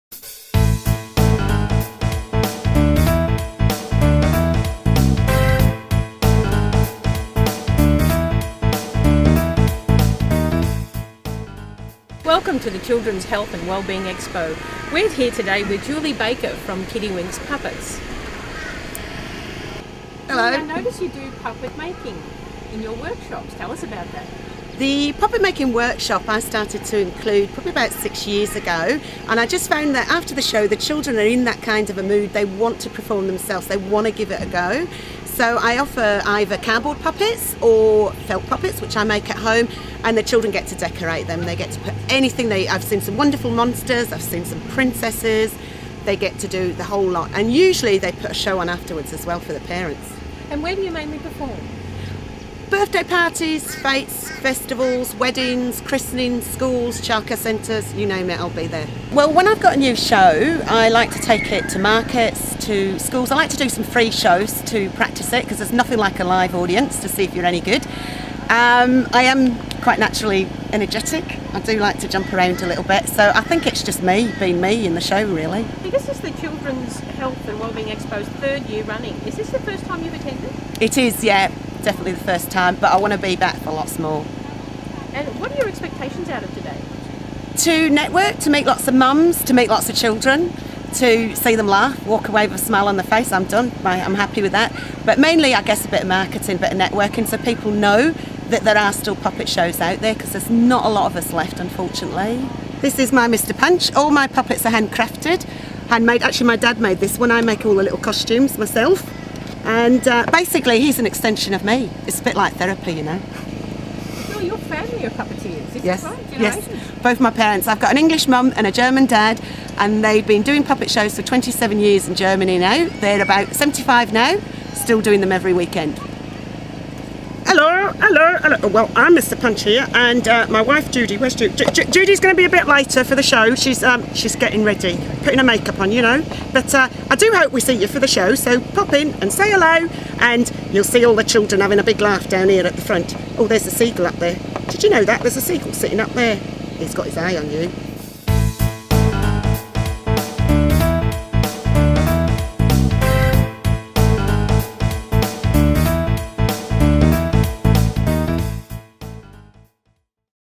Gold Coast Children's Health and Well-being expo Kiddy winks puppet show
Gold coast Children's Health and Wellbeing Expo - Kiddy winks Puppet show, Julia Baker